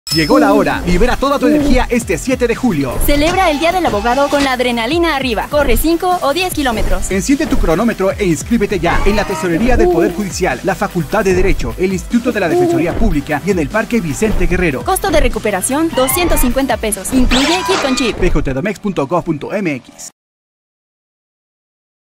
Spot Carrera del Día del Abogado 2019
Participa este 7 de julio en la Carrera2019 del DíaDelAbogado. ¡Juntos llegaremos a la meta!